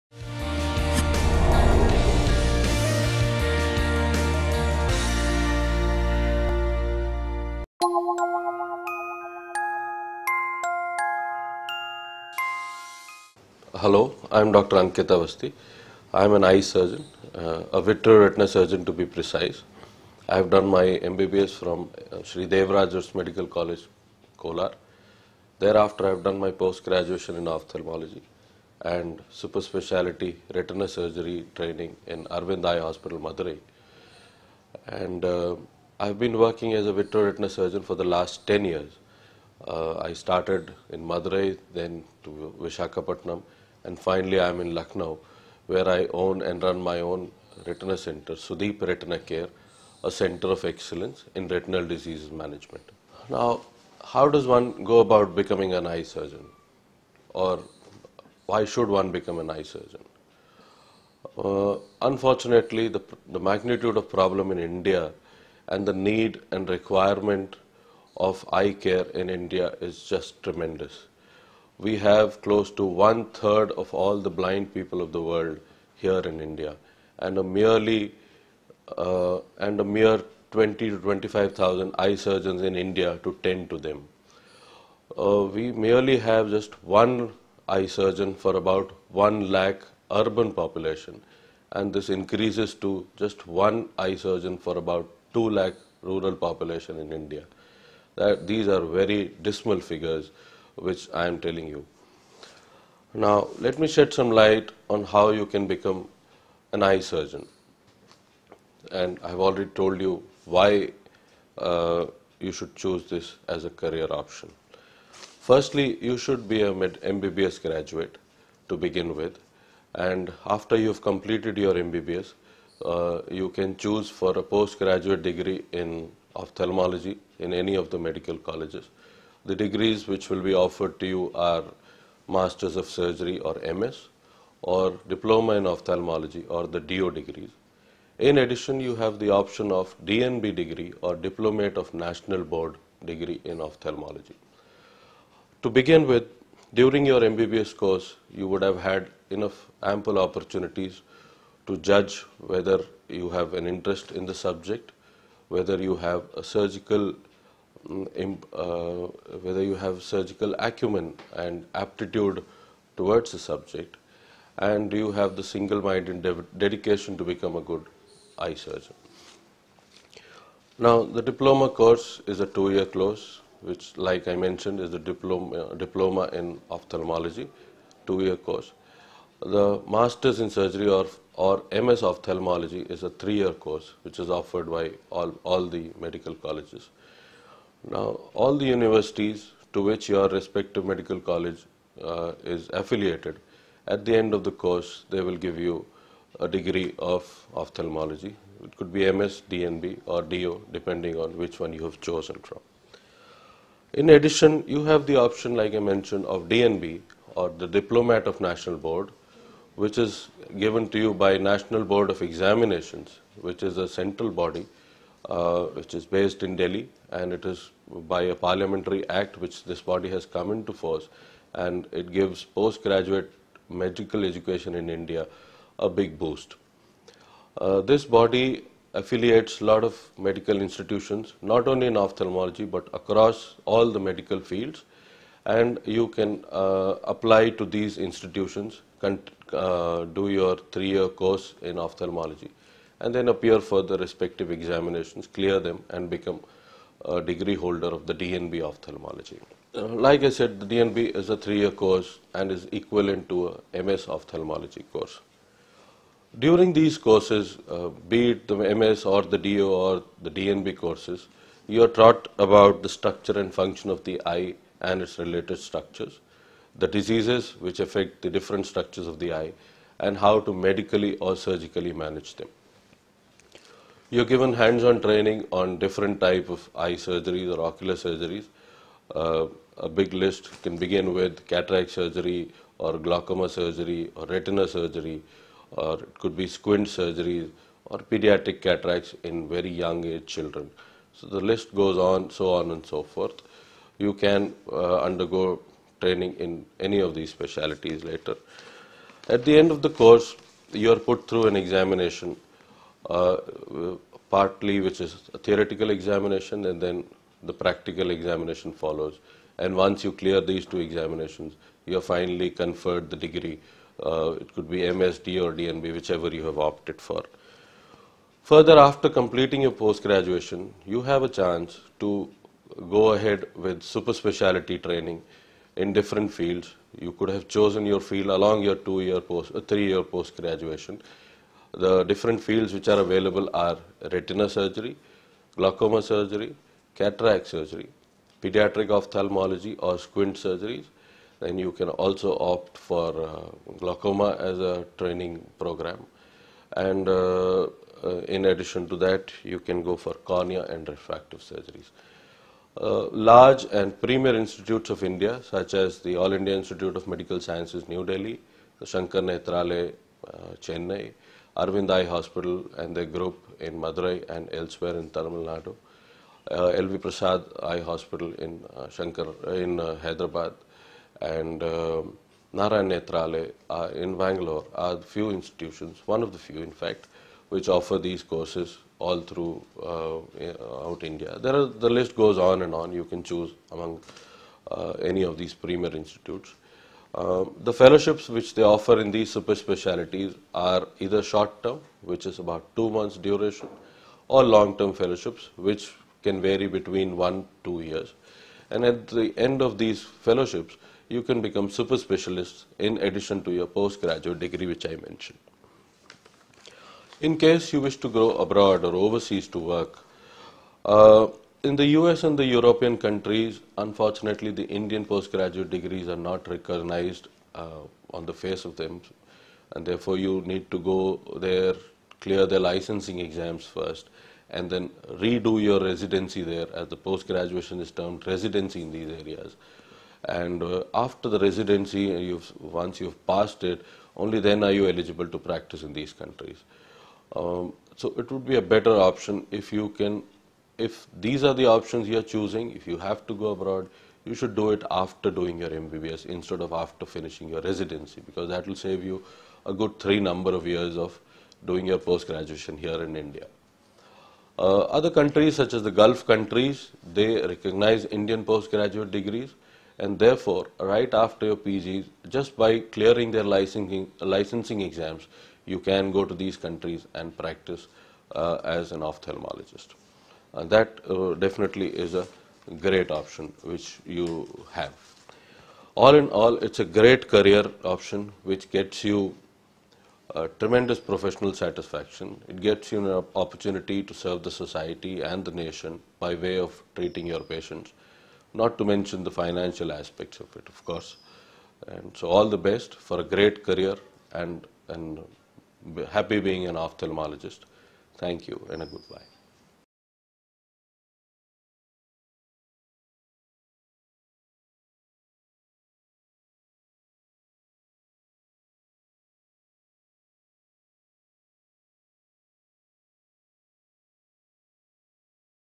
How to become an Eye Surgeon | English
Career in Ophalmology | Insider Tips by Retina Specialist Dr. Ankit Avasthi